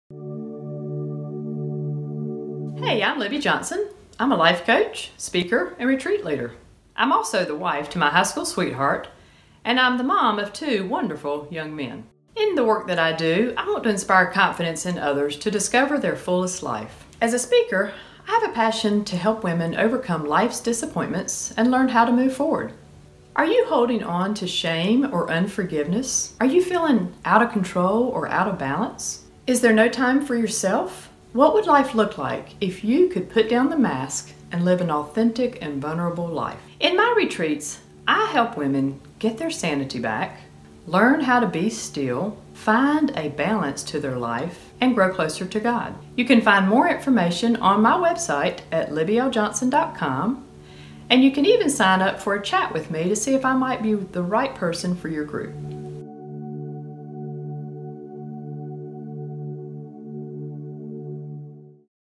Libby Johnson - Public Speaker, Motivator, Preaching
LIbby Johnson blends her coaching experience and professionalism to her public speaking.  If you need a speaker for your event, church service, retreat, or corporate event, you can trust Libby to deliver and motivate.